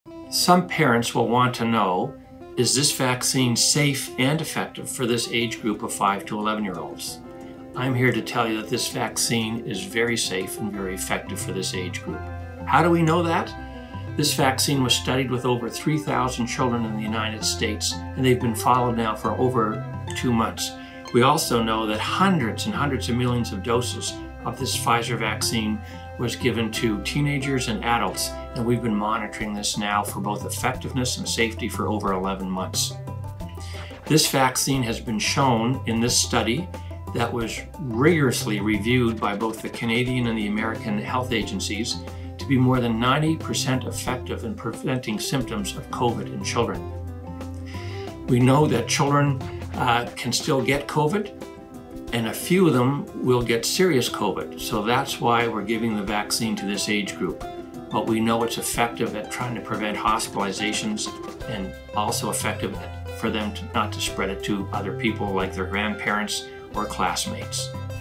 Dr. Andrew Lynk Q&A: "How was the vaccine determined to be safe and effective for children?"
Dr. Andrew Lynk is Chair of Pediatrics at Dalhousie University and Chief of Pediatrics at the IWK Health Centre.

COVID-19 vaccine appointments are open to booking provincewide for children aged 5 to 11.

You can schedule a COVID-19 vaccination appointment for your child online at NovaScotia.ca/Vaccination or by phoning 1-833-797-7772.